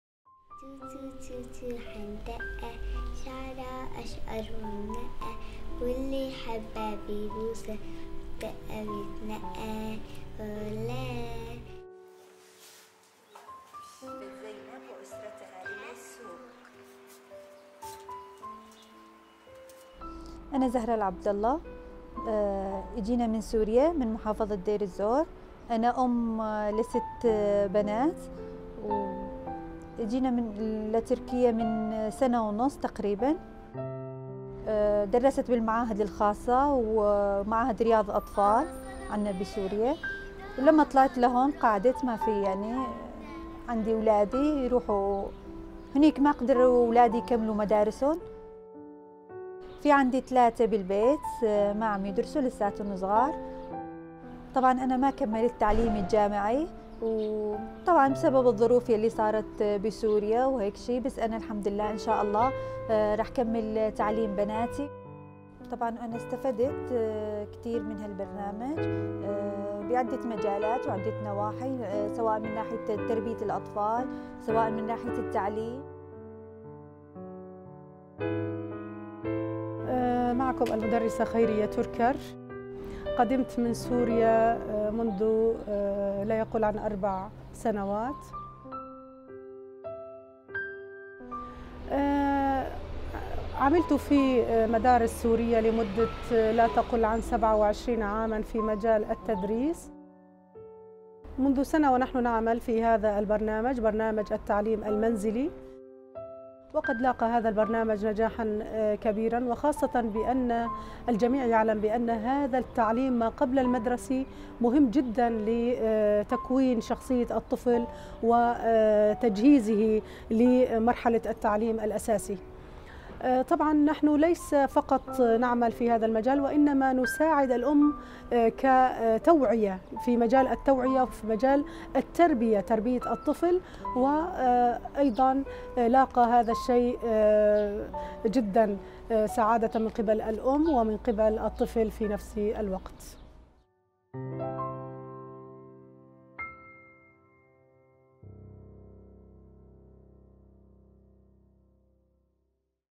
"Home and community-based Early Childhood Services Access the container"
In collaboration with UNICEF in GAP Administration and the "home and community-based Early Childhood Services Access the container" Project promotional video